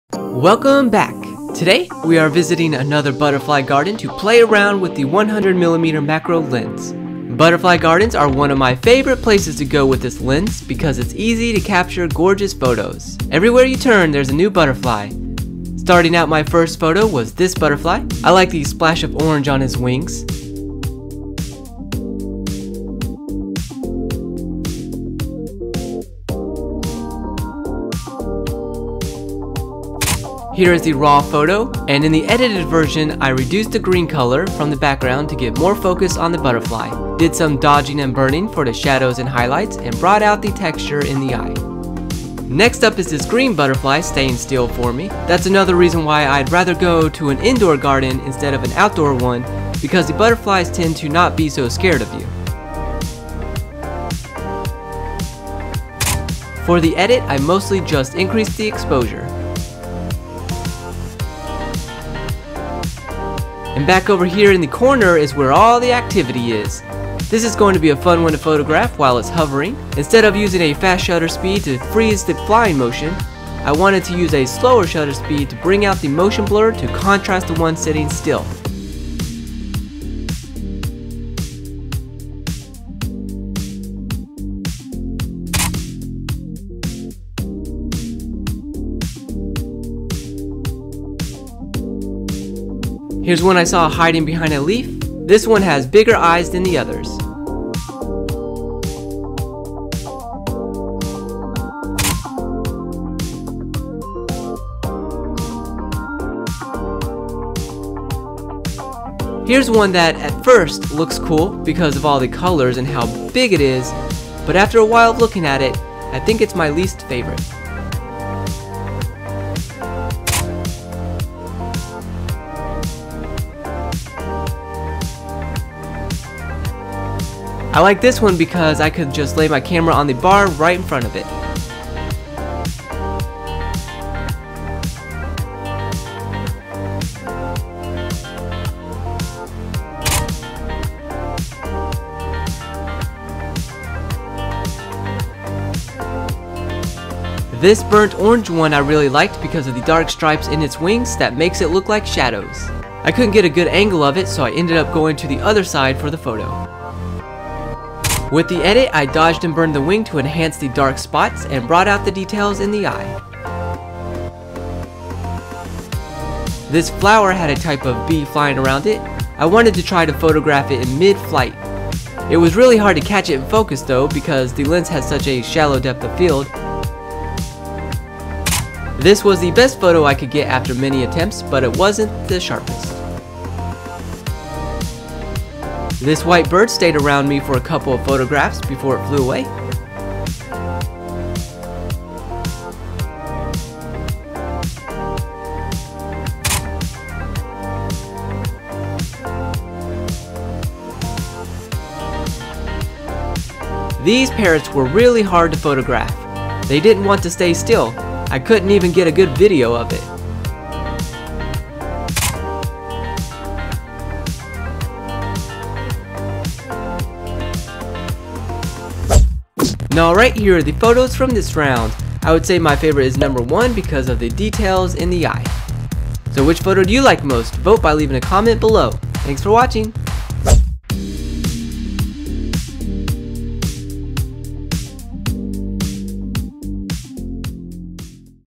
Macro 100mm F2.8L Butterfly Photography.
Today we are visiting another butterfly garden to play around with the 100mm macro lens.